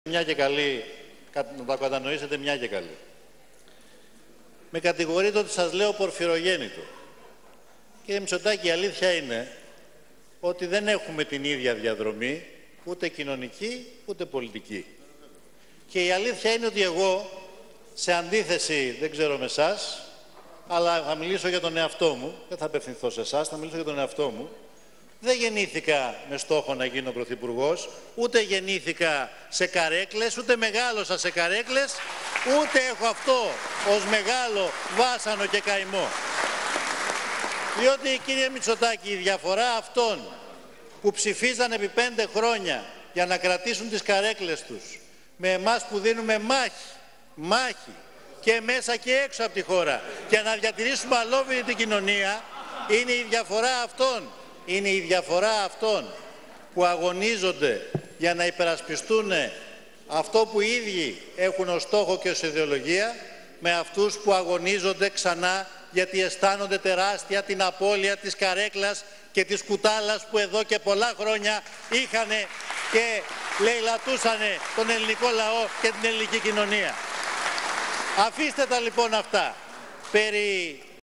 Αλ. Τσίπρας: Με τον κύριο Μητσοτάκη δεν έχουμε την ίδια διαδρομή
«Τι σχέση έχουν αυτοί που έδιναν μάχη για καρέκλες με εμάς που δίνουμε μάχη για τη χώρα;»